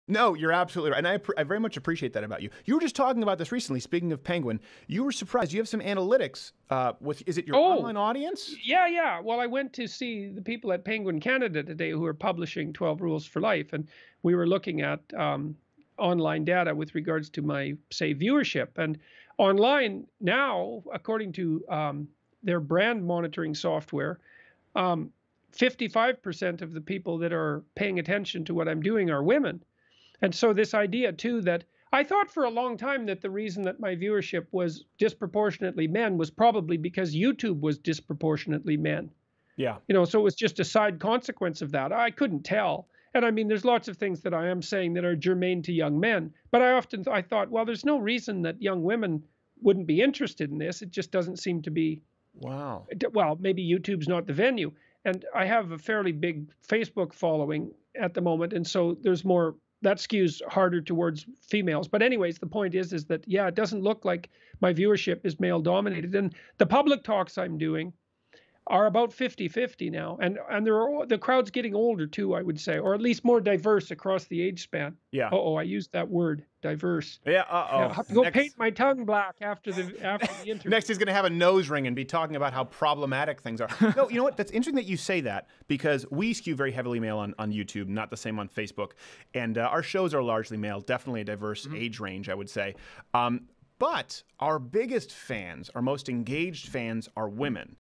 Jordan Peterson Breaks Down the Demographic of His Audience
Jordan Peterson breaks down the diversity of his viewership.